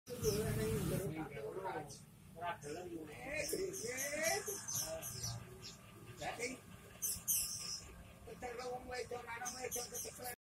Kucing Tertidur pulas
Habis mandi langsung tepar tidur (Bonjel)